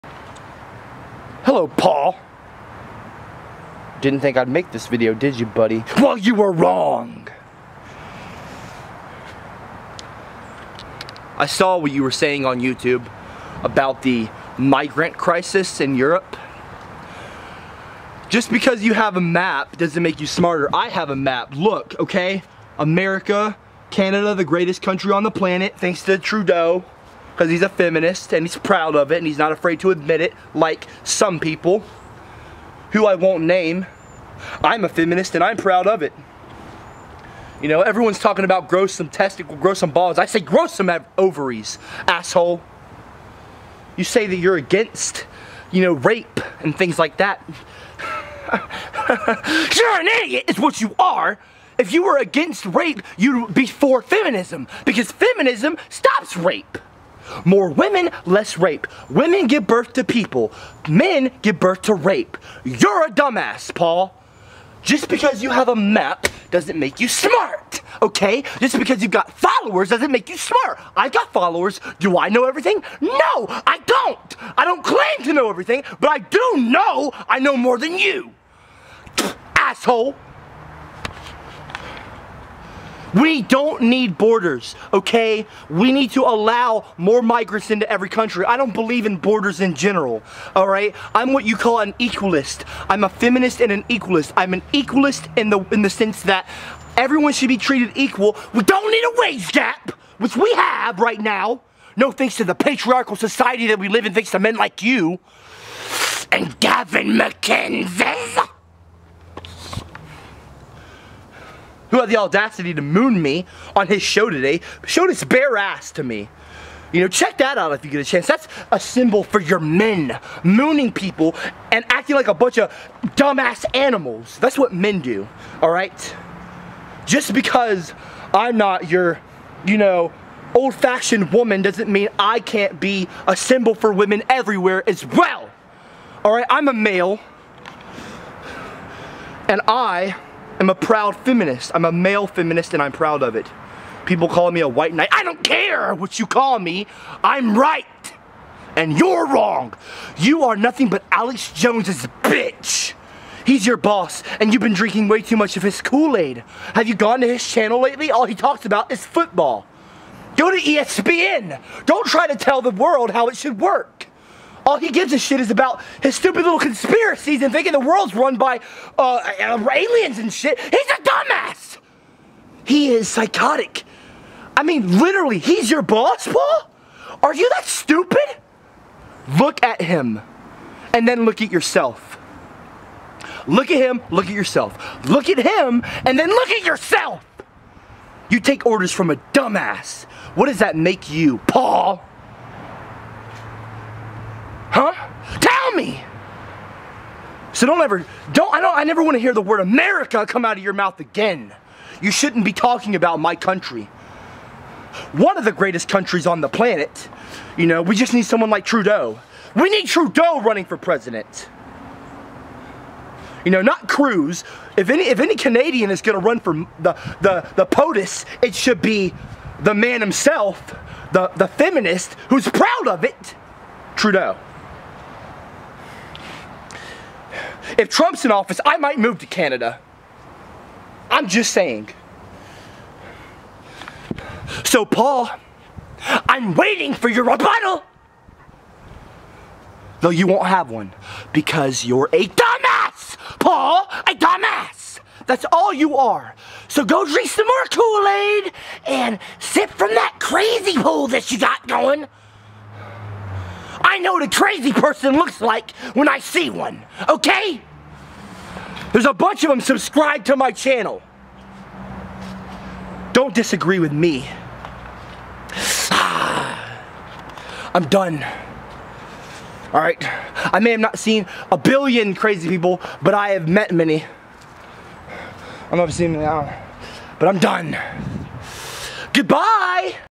FUCK PAUL JOSEPH WATSON!!
PAUL JOSEPH WATSON AND ALL OF HIS FOLLOWERS ARE A BUNCH OF RACIST MALE CIS BIGOTS WHO WANT NOTHING MORE THAN TO BRAINWASH THE REST OF THE INTERNET INTO BELIEVING THERE LIES... HERE'S HIS CHANNEL

PAUL, IF YOU'RE READING THIS... PLEASE, LISTEN TO ME. QUIT TAKING ORDERS FROM THAT NUT JOB ALEX JONES... YOU'RE ALL TOOOO FAR GONE I SUPPOSE. BUT HEY I GUESS I TRIED.

ALSO... GAVIN MCINNES MOONED ME ON LIVE BROADCAST SO I MOONED HIM BACK HE'S NOTHING BUT AN ANIMAL WHO HAS NO FACTS TO BACK UP HIS ARGUMENT HE JUST YELLS INTO THE MIC AND PRETENDS TO BE RIGHT... IF YOU WANT TO WATCH HIM YOU CAN SUBSCRIBE TO THE ANTHONY CUMIA NETWORK, BUT I DON'T RECOMMEND IT.

THERE'S THE LINK.

PEACE!